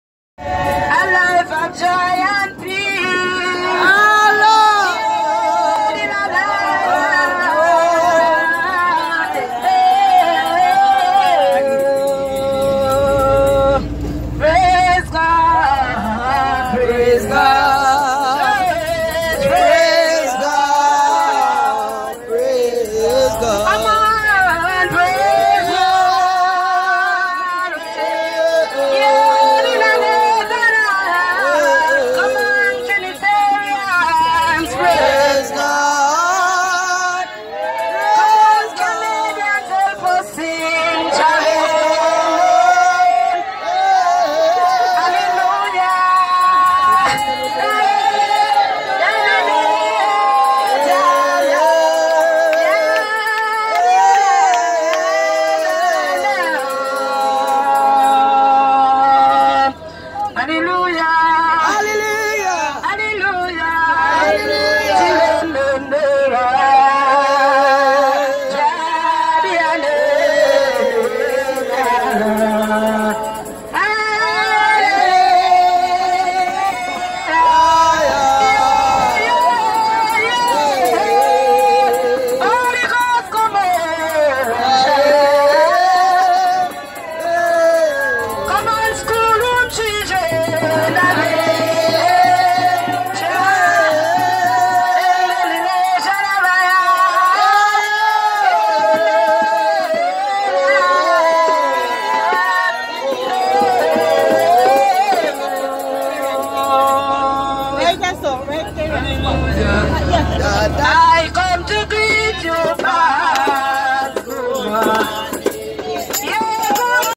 48 Mount Refuge HolyTrinity Zion church..
On our way to Watt town singing the Hymns of Zion.. March 5.2020.